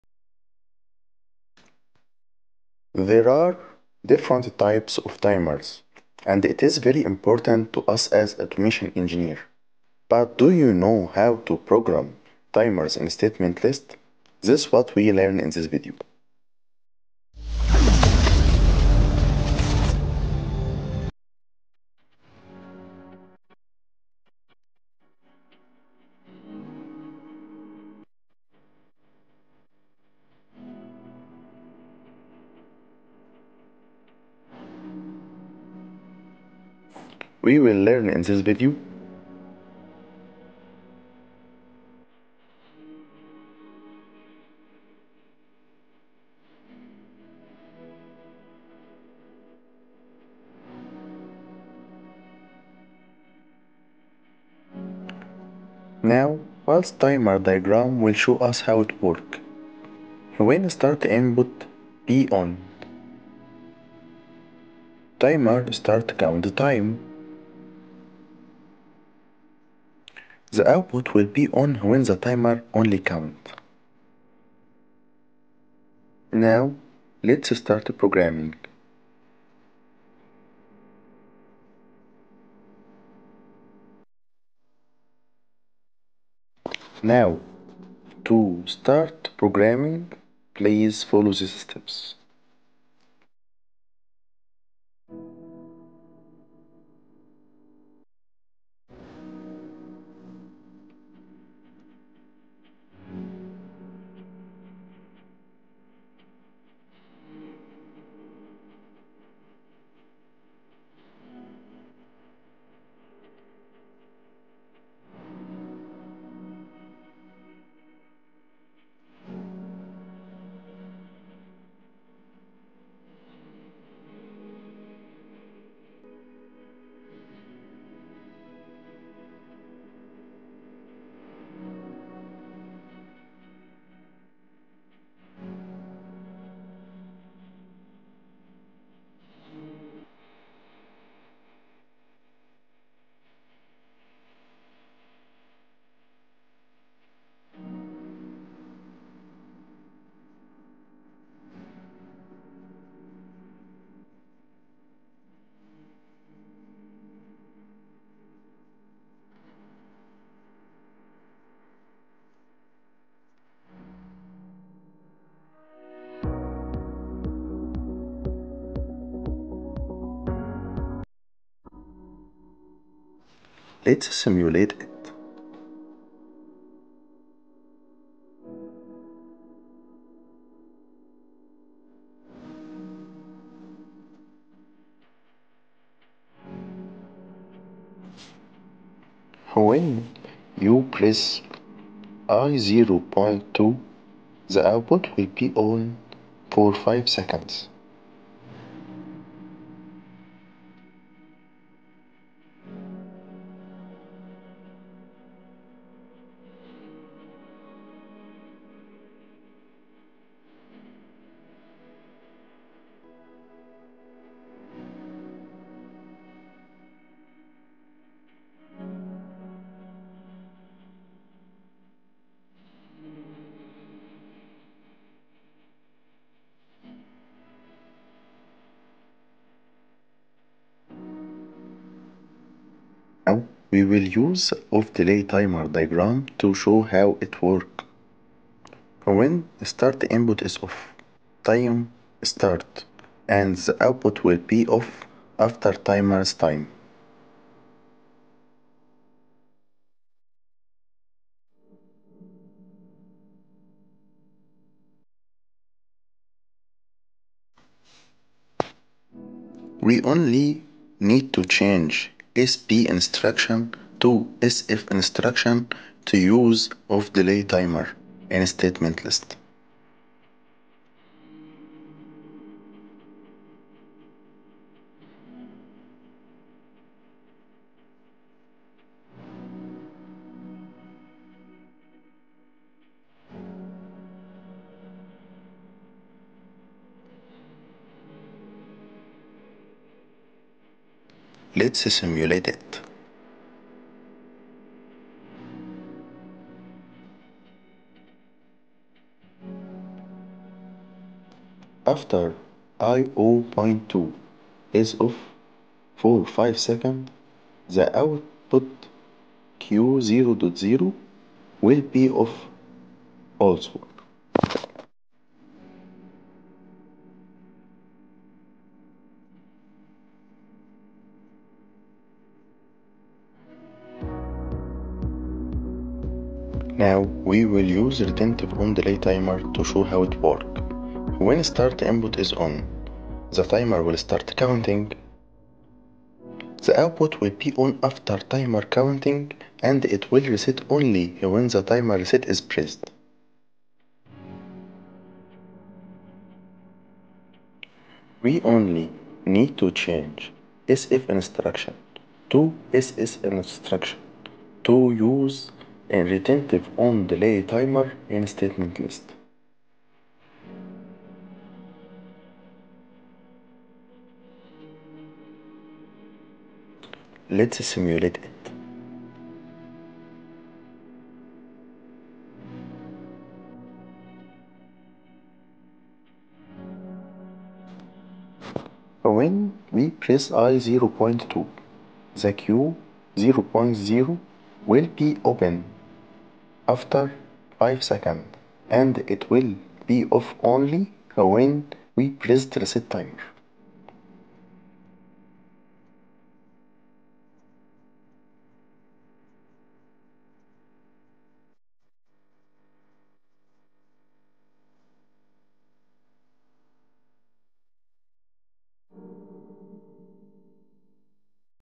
(statement list course) different types of timers by STL | lesson 3 simatic manager
how to use different types of timers in STL
what are the different types of timers
how to simulate them